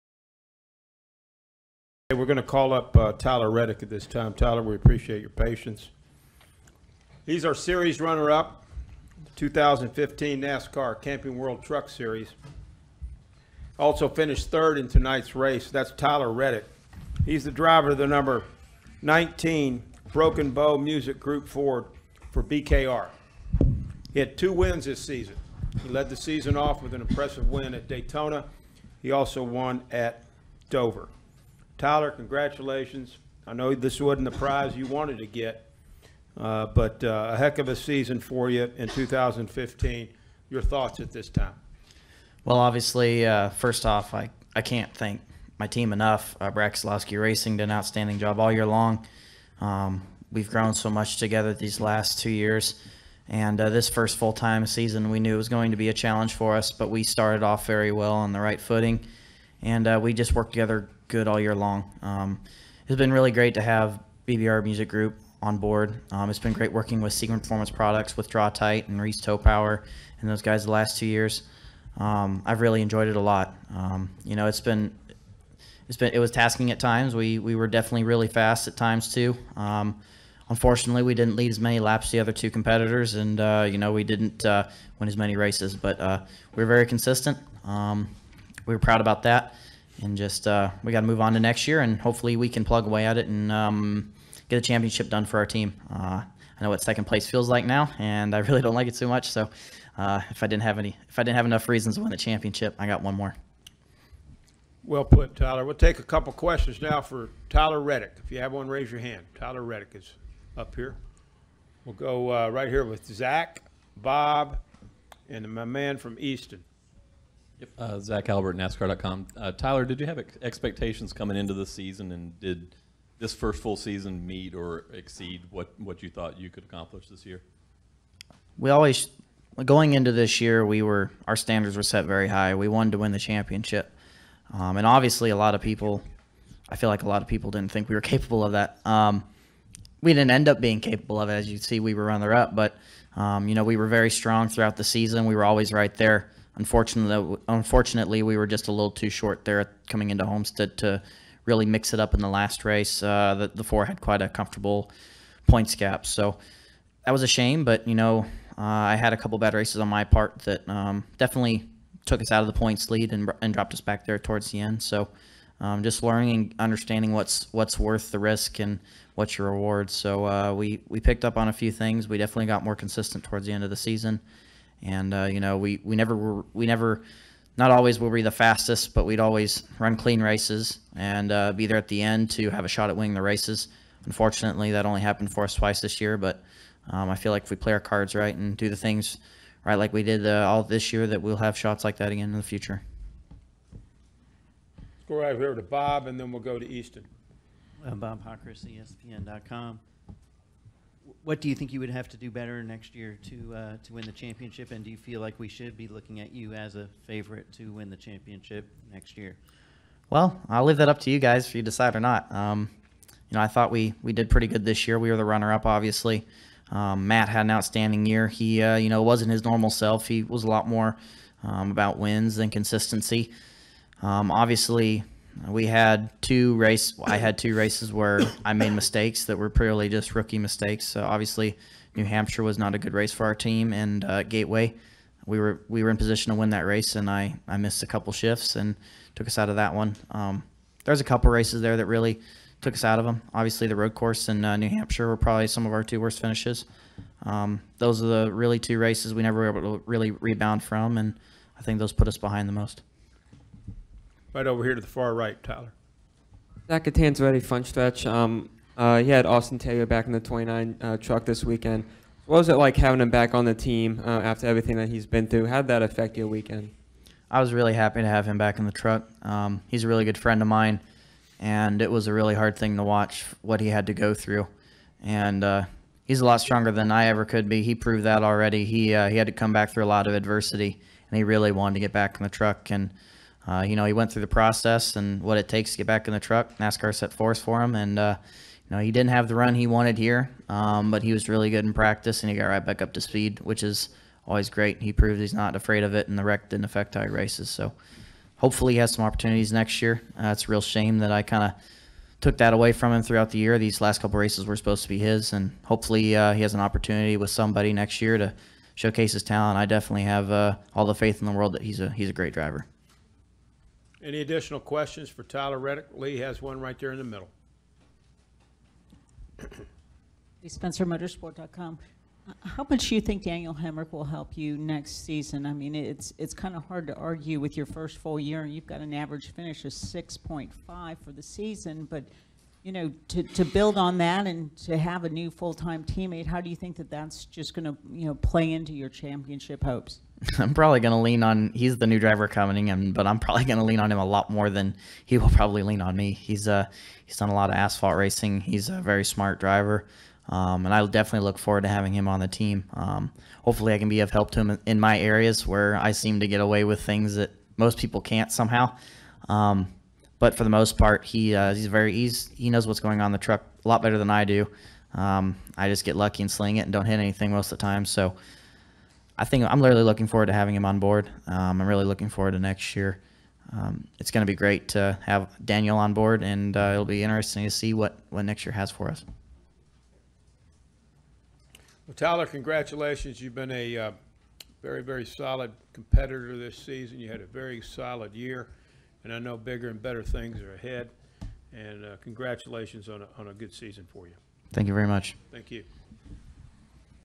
Tyler Reddick HMS 2nd in Points, 3rd in Race Interview
Tyler Reddick talks about his determination in finishing 2nd in the final NCWTS point standings.